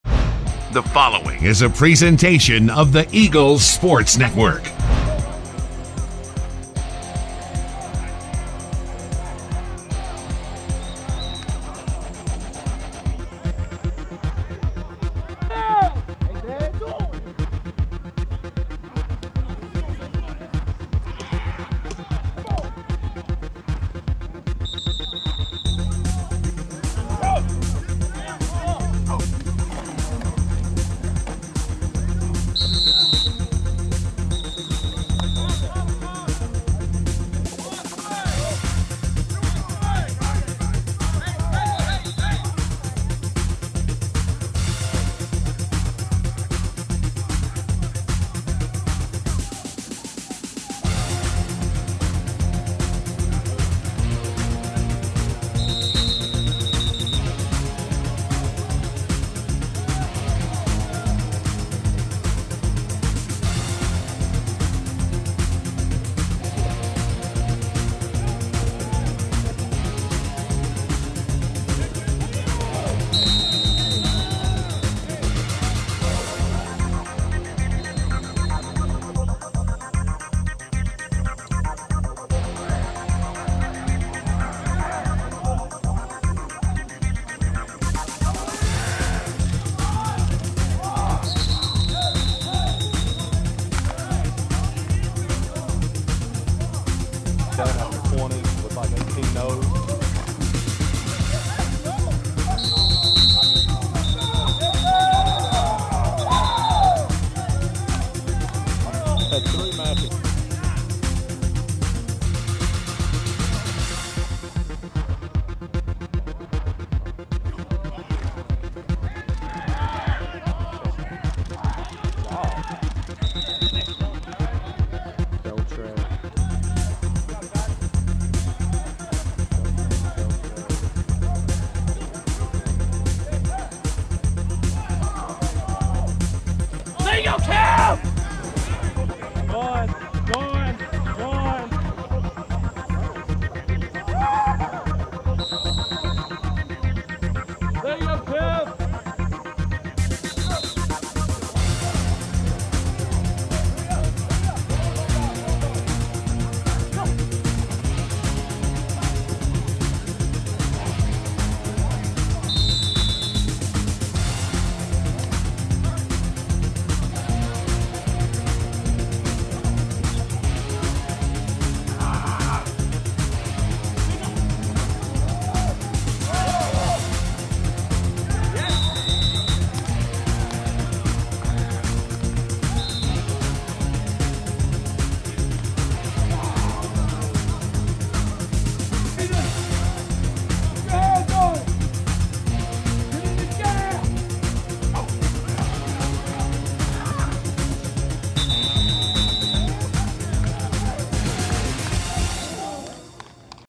Carson-Newman Football 2016: Media Day Scrimmage Highlights 8-17-16
Highlights from Carson-Newman football's first scrimmage of 2016 camp. Watch as Dorren Miller hauls in a pair of passes for scores.